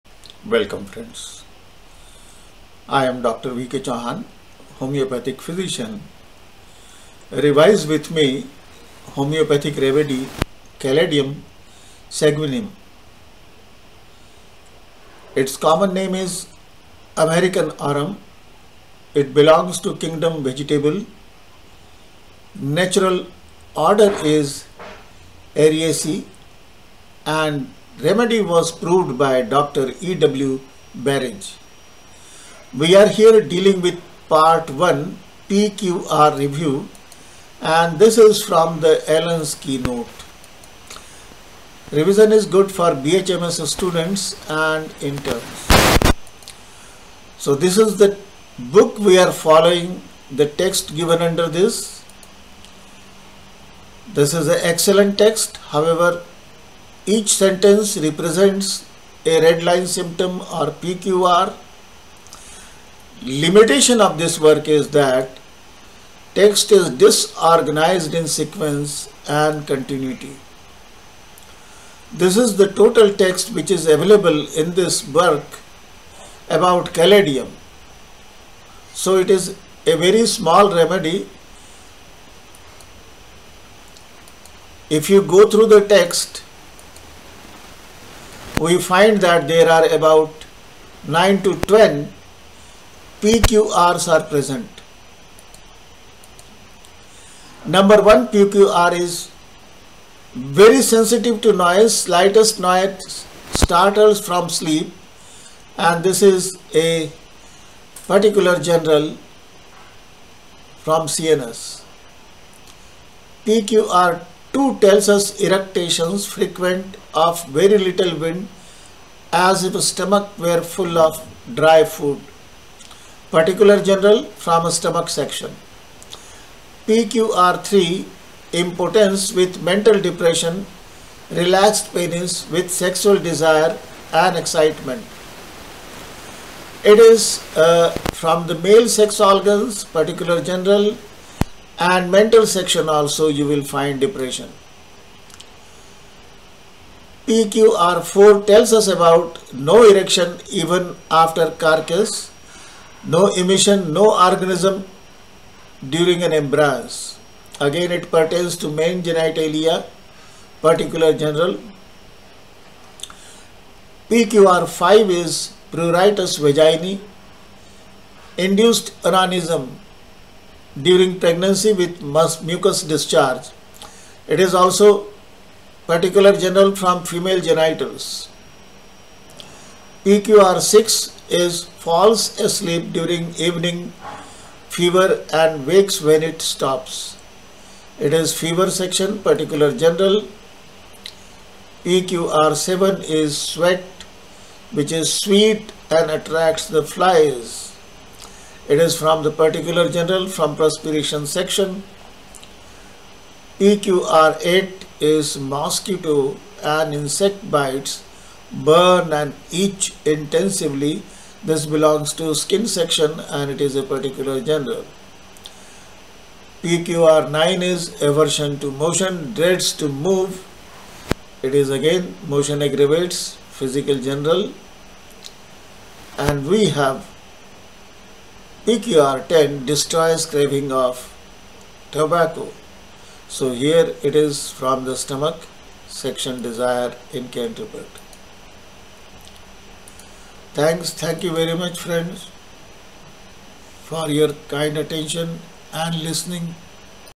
41. Part-1. PQR Review Caladium Seguinum 210423
The Remedy is reviewed from Allen's Keynote, into its important PQRs and their background.